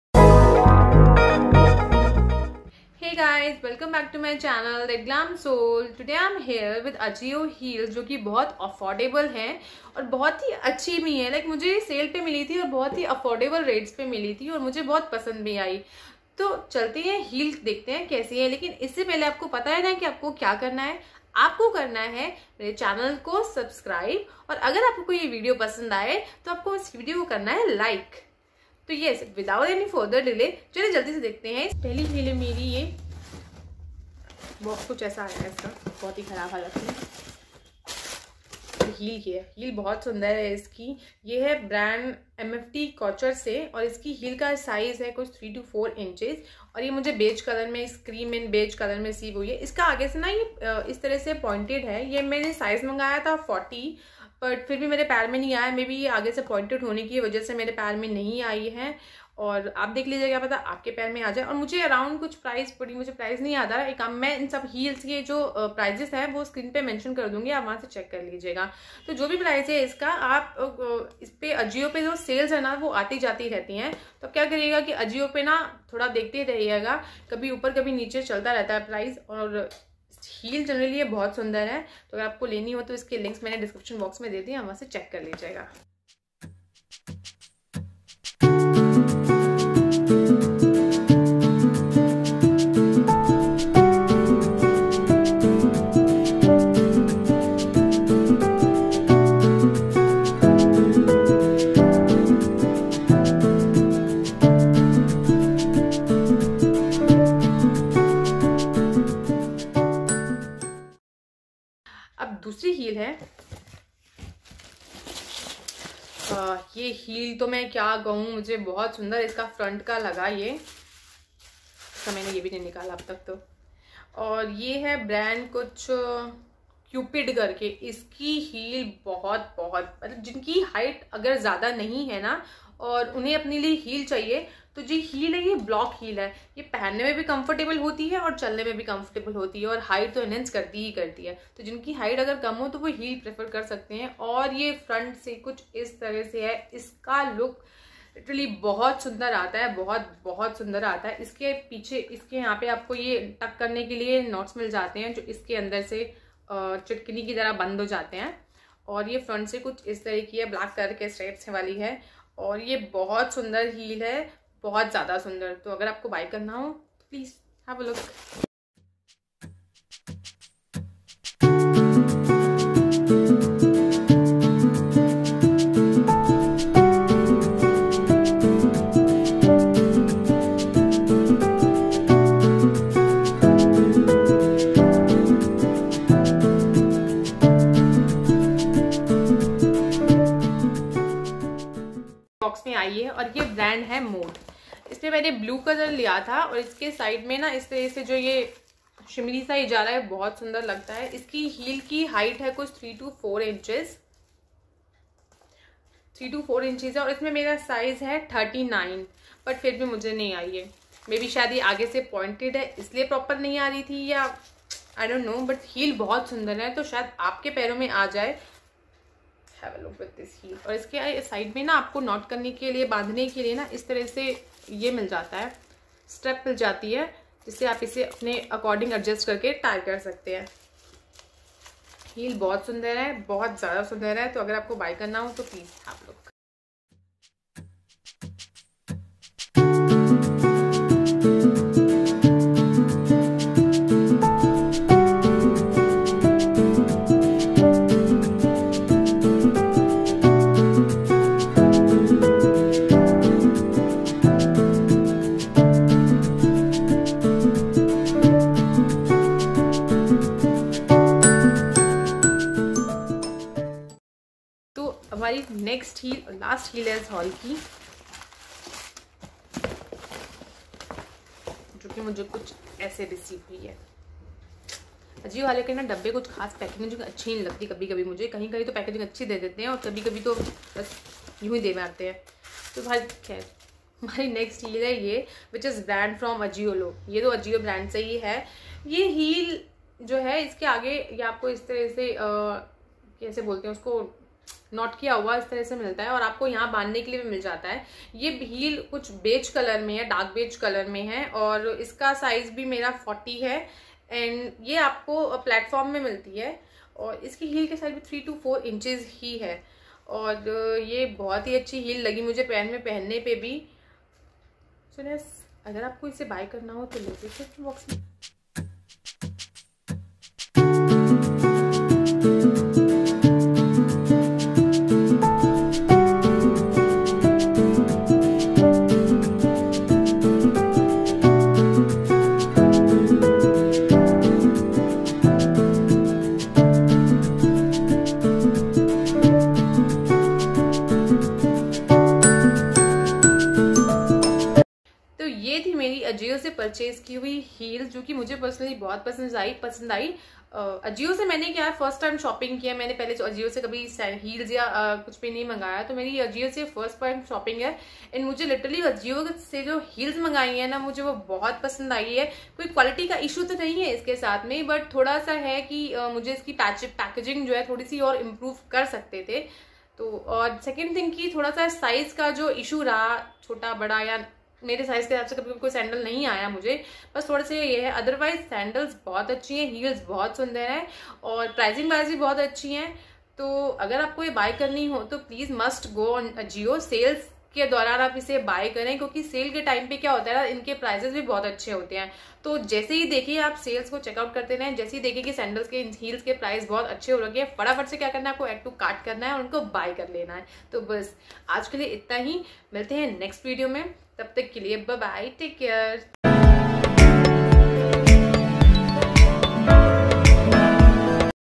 ||Ajio Footwear Haul || Classy & Trendy Footwears || Try-on & Review||
Today's video is a " 👠Ajio Footwear Haul  | block Heels | & More "

I have bought some amazing stuff which will keep you stylish and comfortable.

Product Link:
1) MFT COUTURE textured pointed-toe pumps (out of stock)
2) QUPID chunky heeled Gladiator sandals (out of stock)

Love…. AARTI  ❤❤❤❤❤❤❤❤